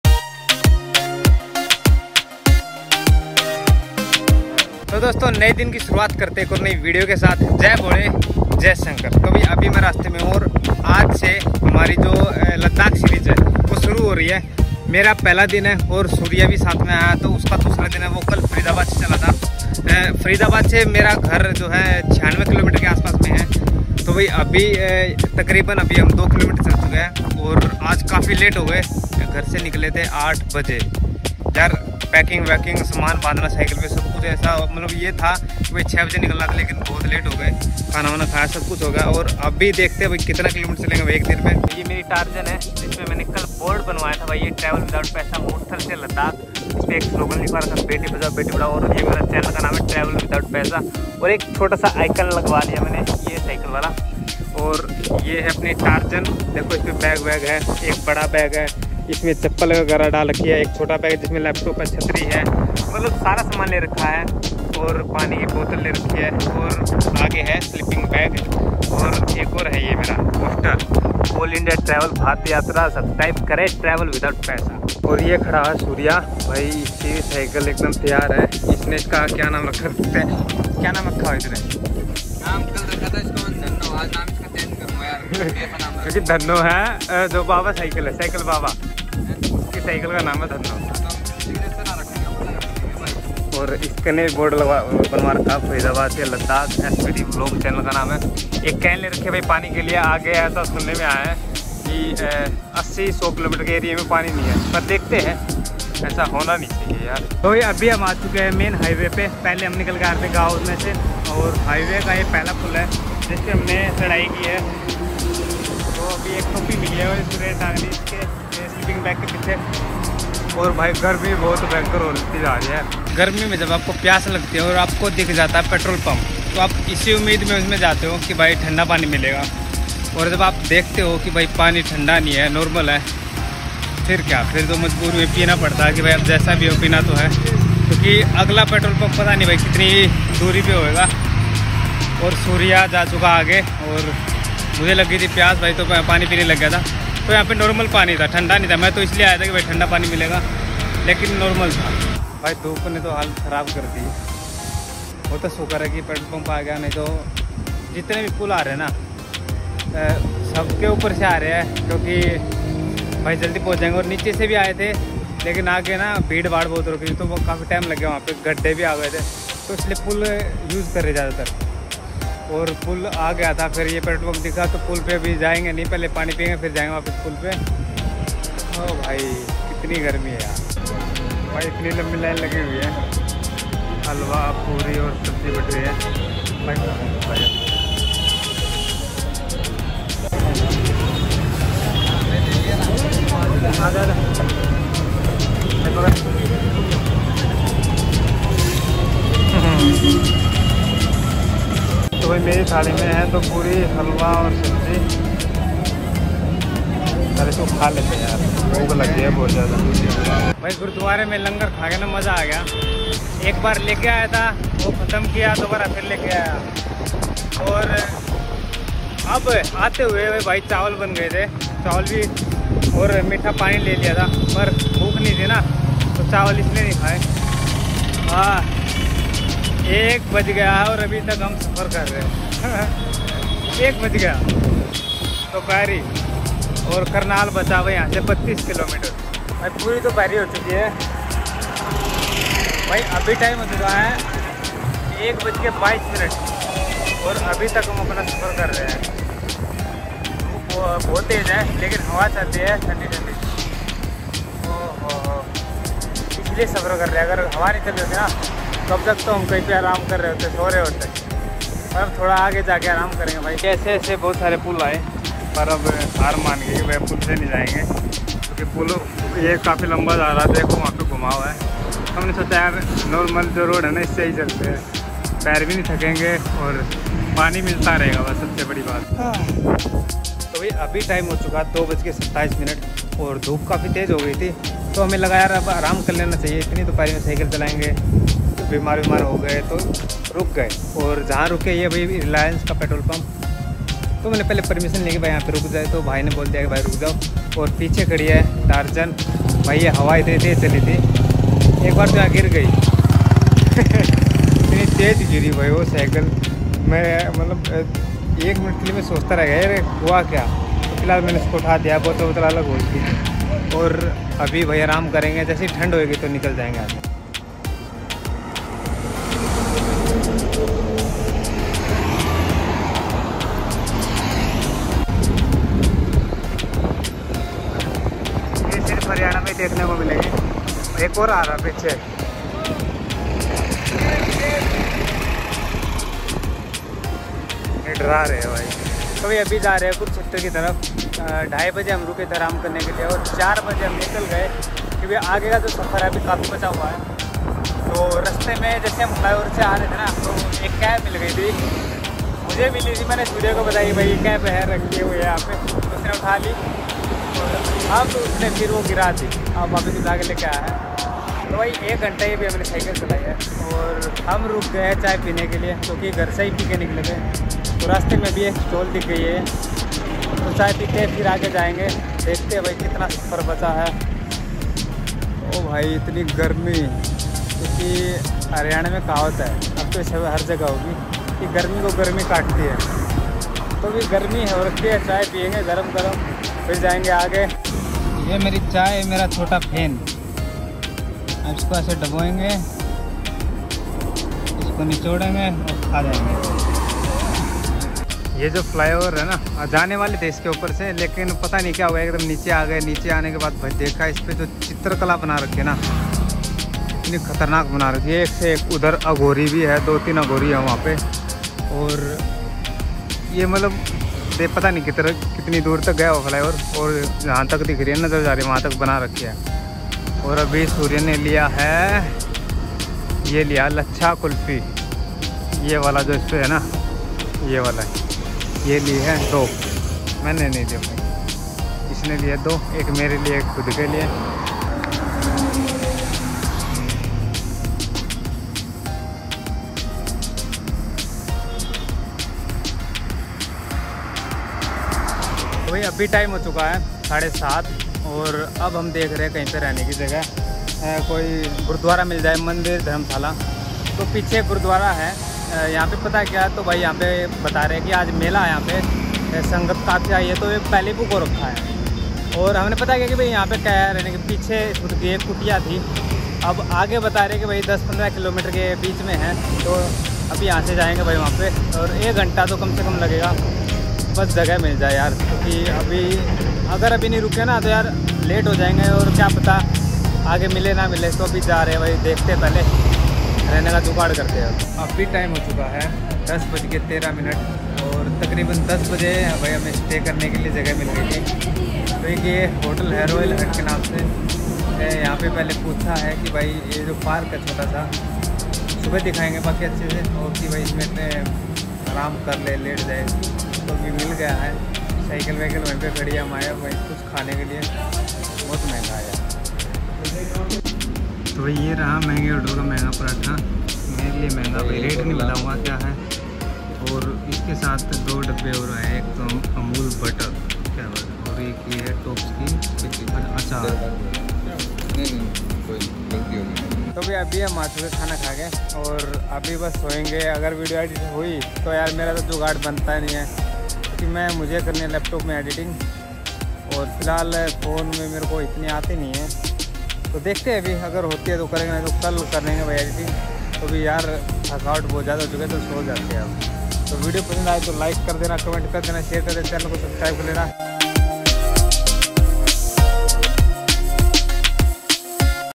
Ladakh Road Trip by Cycle🚲 | Murthal to Ladakh Cycle Ride 2022 | Day-1 | Ladakh Vlog 2022
For Help - UPI ID ( radhe0867@okicici )
thnks for your help 😍❤️😘

Thank You Help for Travel 😍😘